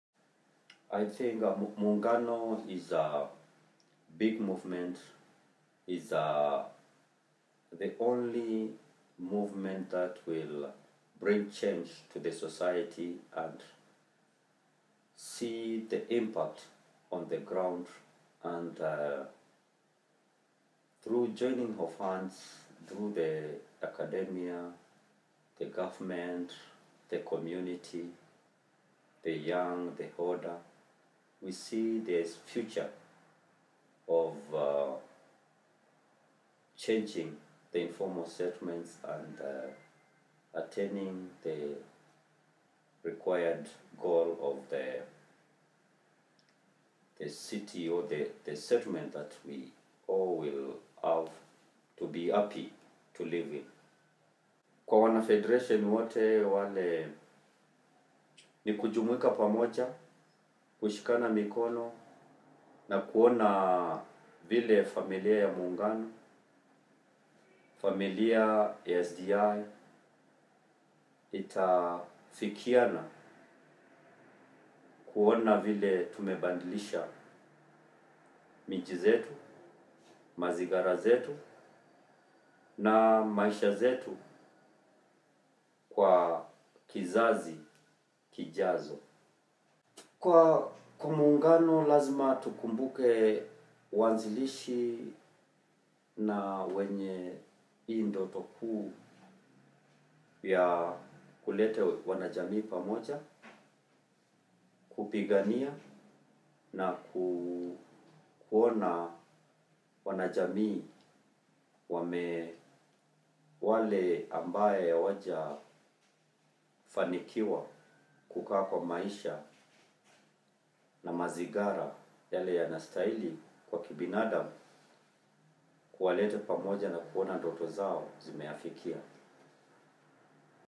Rashid Mutua 10/10: What are your hopes for Muungano’s next 20 years?
This interview is with:

Rashid Mutua
Muungano
From: Bawazir, Athi River, Machakos
Interview: 8 April 2016, Nairobi
Interviewer(s): Asha Ali & Kate Lines
Original language: English & Swahili (English subtitles)